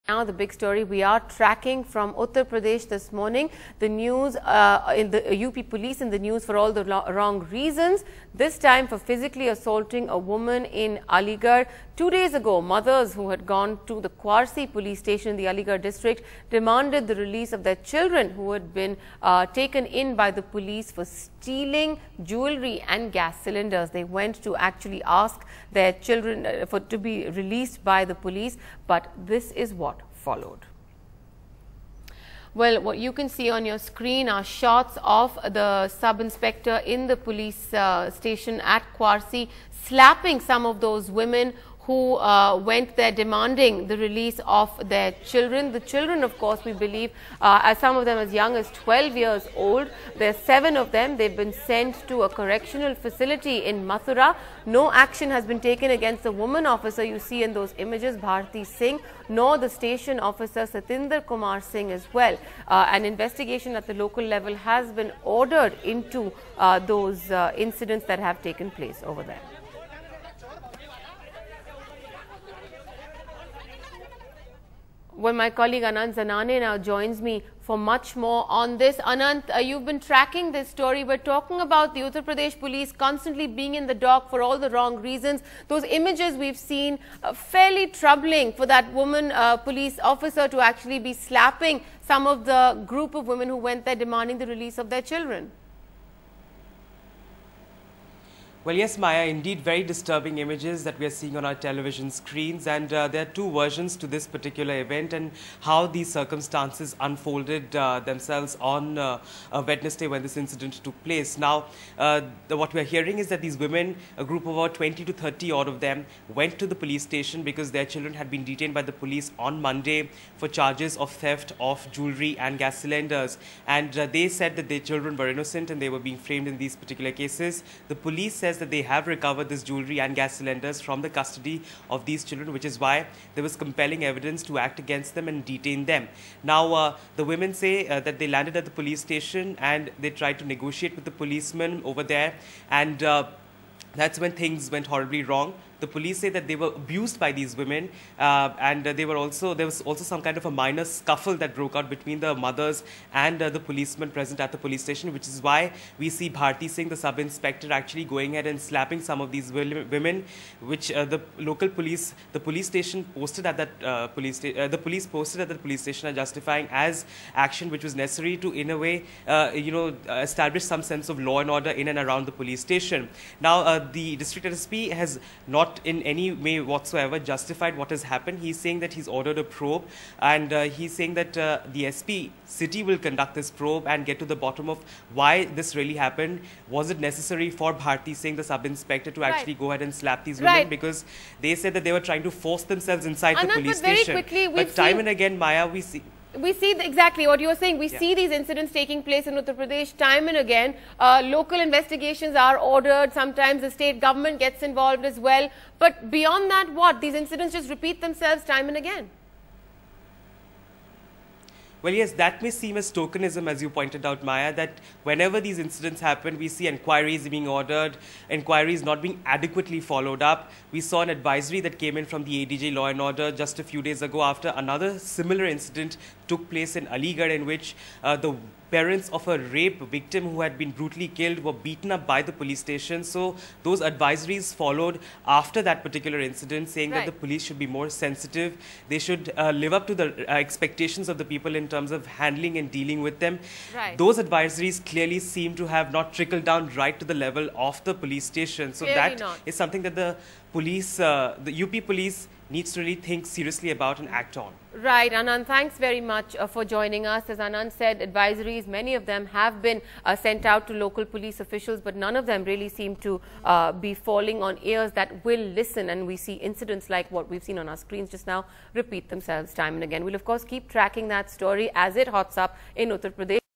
Now, female cop beats up women in Uttar Pradesh
Bharati Singh, a woman cop in uniform says she was asked by her superior officer to slap several women at an Aligarh police station in Uttar Pradesh. On camera, Ms Singh is seen raining blows on the women, who had gathered to demand the release of their children detained in a case of theft.